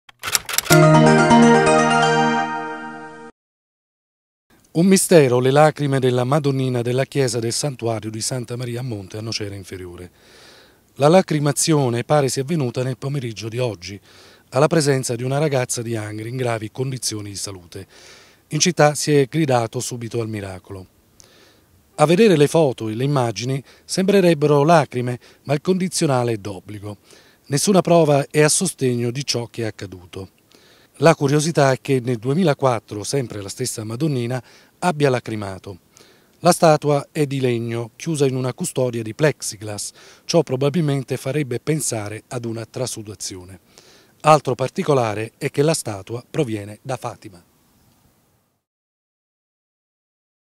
Nocera Inferiore. La madonnina di Montalbino in lacrime: miracolo?
Un mistero le lacrime della madonnina nella chiesa del santuario di Santa Maria a Monte a Nocera Inferiore.